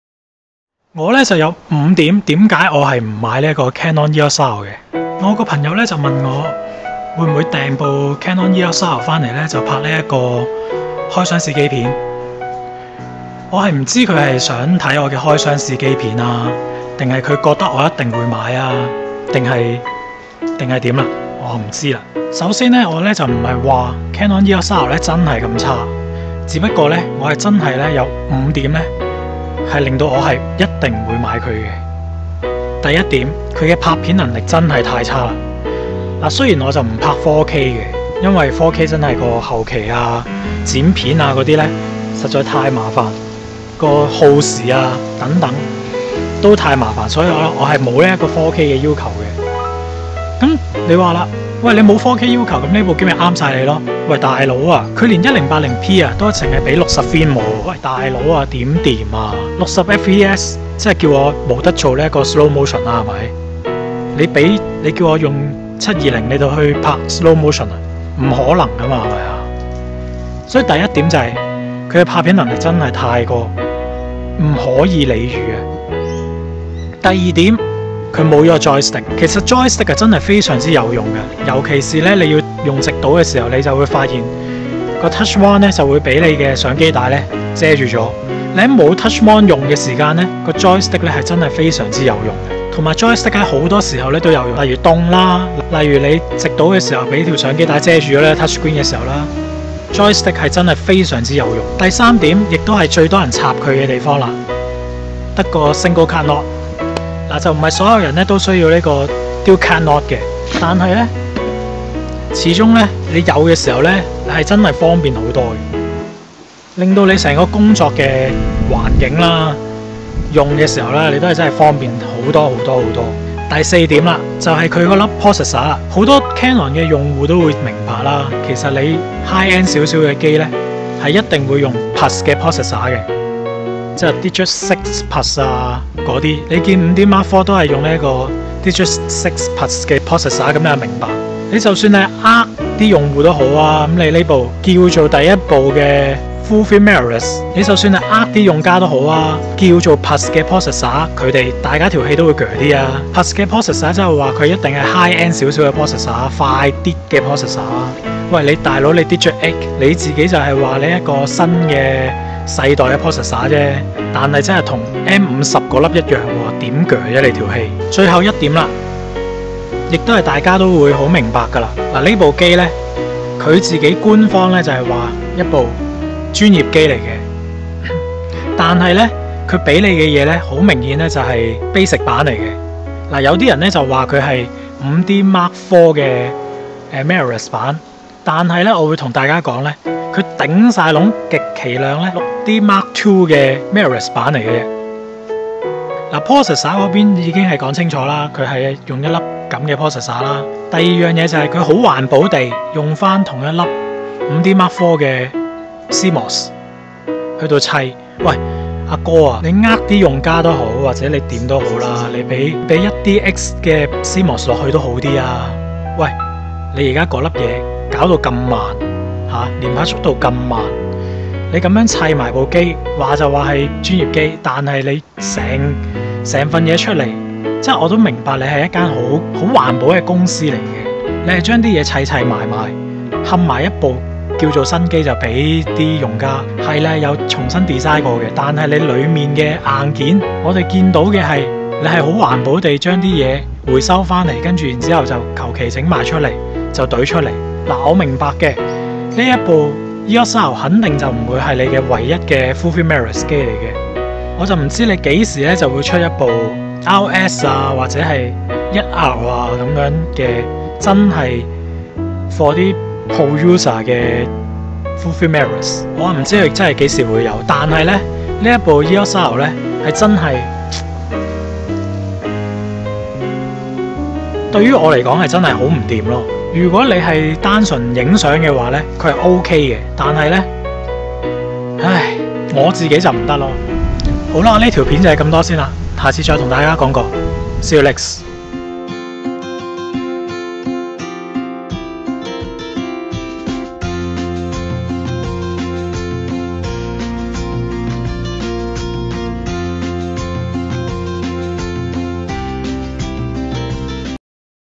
My 5 reasons don't buy Canon EOS R | ENG subtitle 廣東話
我的5個理由唔買Canon EOS R